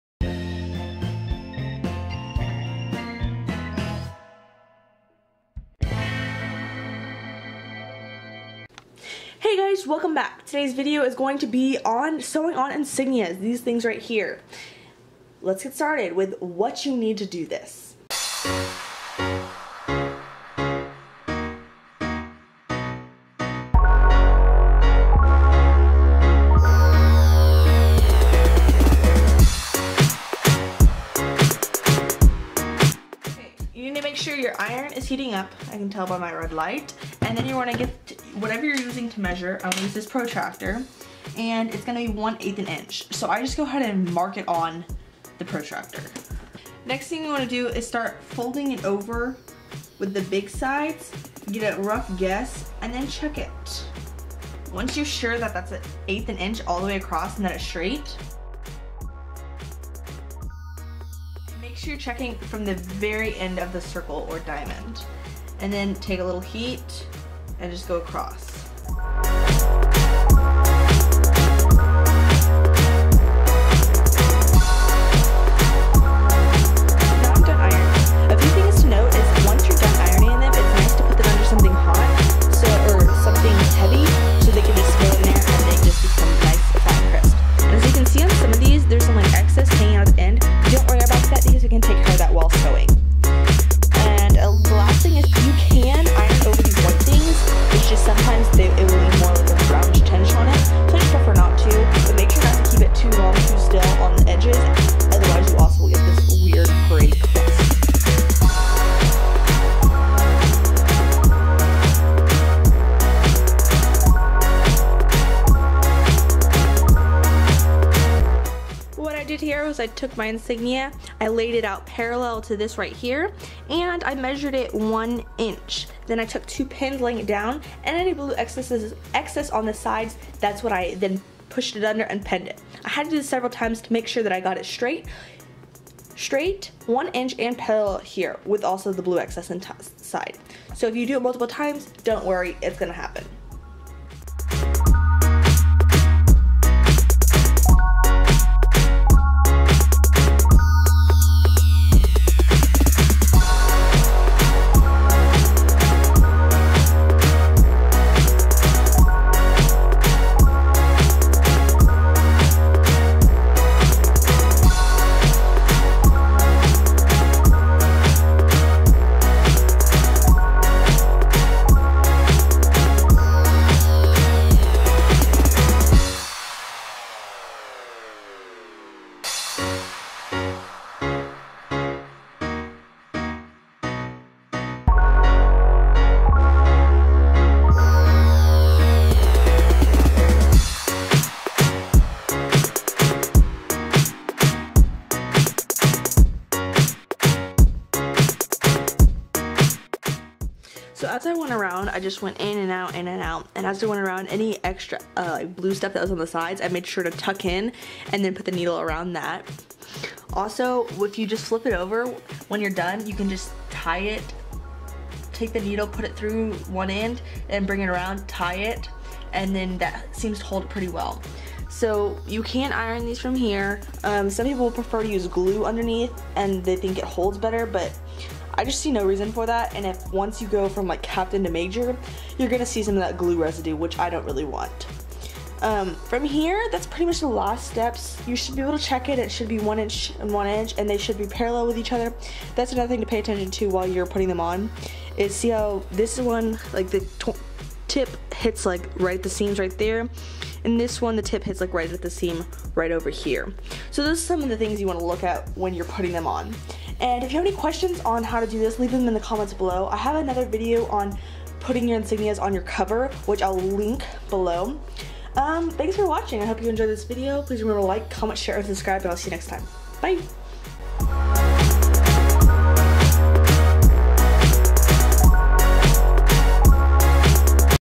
How to Sew on Insignias/Patches/Badges
Do you need custom challenge coins?

Would you like custom made military patches?

Blog Post about Sewing on Insignias!

Question:  Where does cadet officer grade insignia go on the Battle Dress Uniform and the Corporate Field Uniform?

CAPM 39-1 (26 June 2014) states, "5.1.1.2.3. Cadet officers center regular size, full color embroidered insignia on ultramarine blue cloth sewn on the collar one inch from the front of the collar and centered. All insignia will have 1/8 inch of blue showing at the widest and tallest point of the insignia. Cadet Colonels and Cadet Captains may wear miniature, embroidered insignia."

Figure A5-6 appears to show the 1" measurement from the edge of the collar to the edge of the blue cloth, which is 1/8" from the insignia.

However, the measurement is 1" from the front of the collar to the insignia itself, or 7/8" from the front of the collar to the blue cloth.